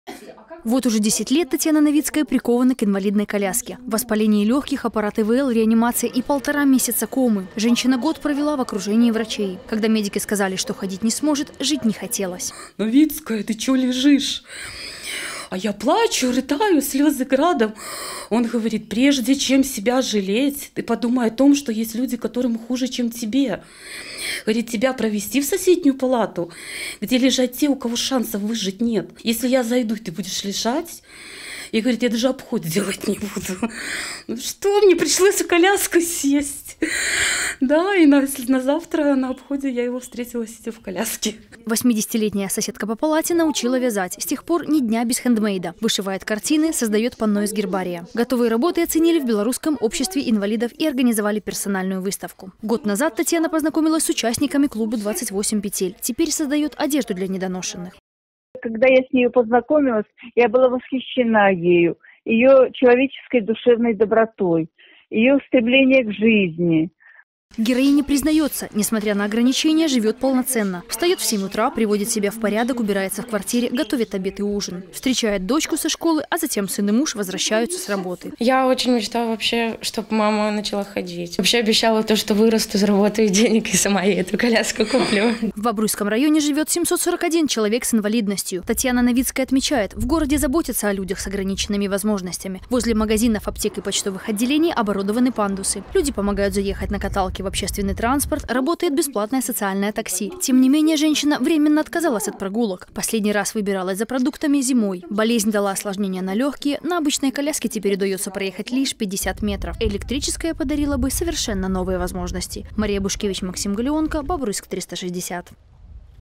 "ПОСЛЕ ПЕРВЫХ ПРОГНОЗОВ, ЖИТЬ НЕ ХОТЕЛОСЬ". ПОЧЕМУ ИНВАЛИДНОСТЬ - НЕ ПРИГОВОР
Оказались в коляске, но не опустили руки. В Бобруйском районе живёт свыше 700 человек с инвалидностью. Мать, жена, героиня.  Татьяна Новицкая признаётся, несмотря на ограничения, живёт как все.
Мария Бушкевич узнала, как ей это удается?